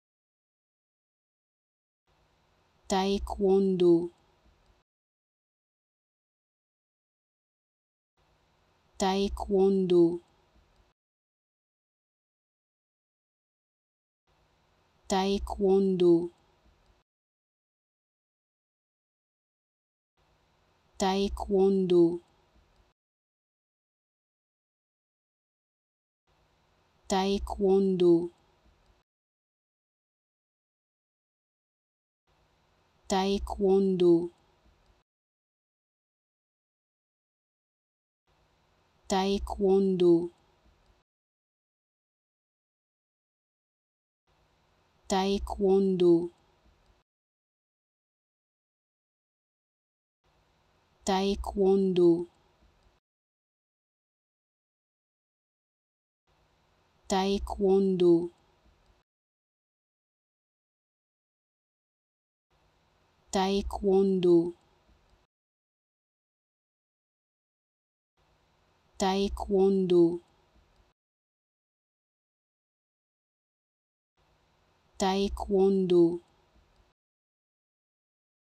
How to Pronounce ''Taekwondo'' Correctly in French
Learn how to say and properly pronounce ''Taekwondo'' in French with this free pronunciation tutorial. Apprenez à prononcer des mots en français grâce à nos tutoriels.